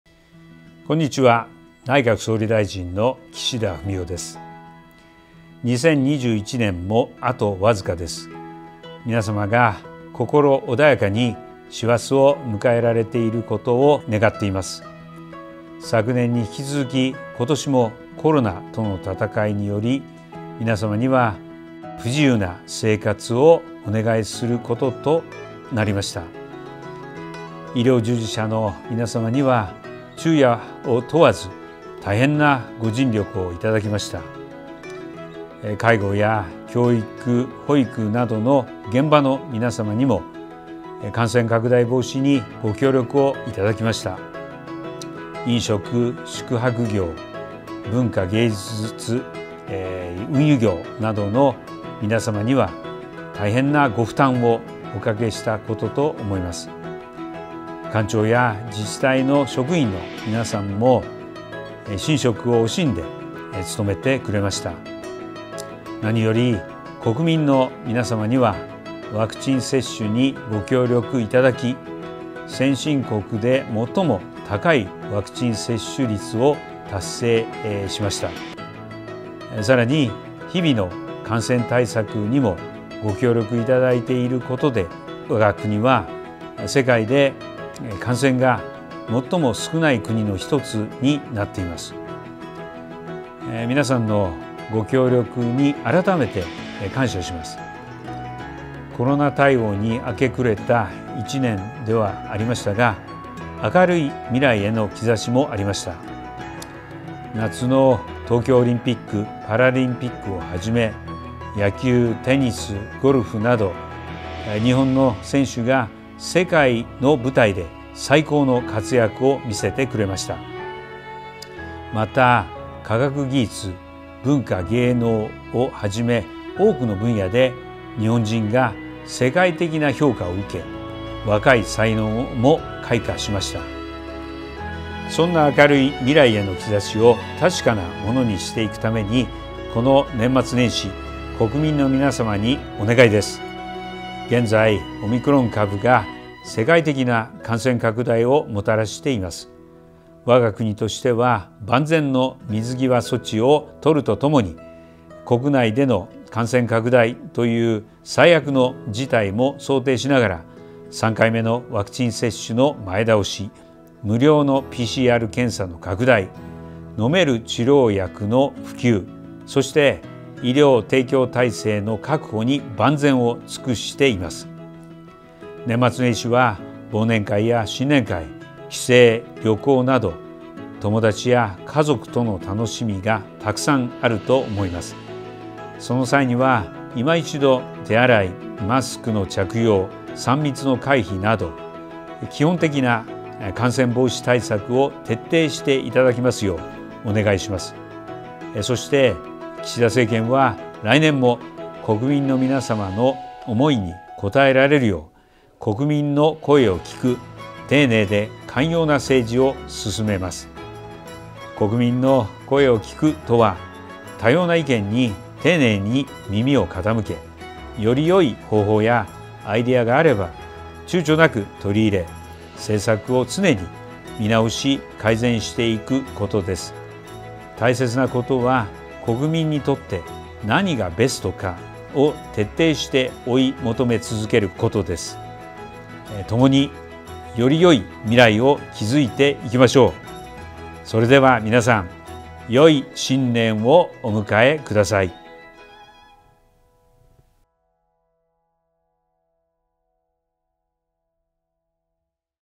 今年一年を振り返っての岸田総理からのメッセージ
年の瀬を迎え、今年一年を振り返り、国民の皆様にお伝えしたいメッセージをまとめました。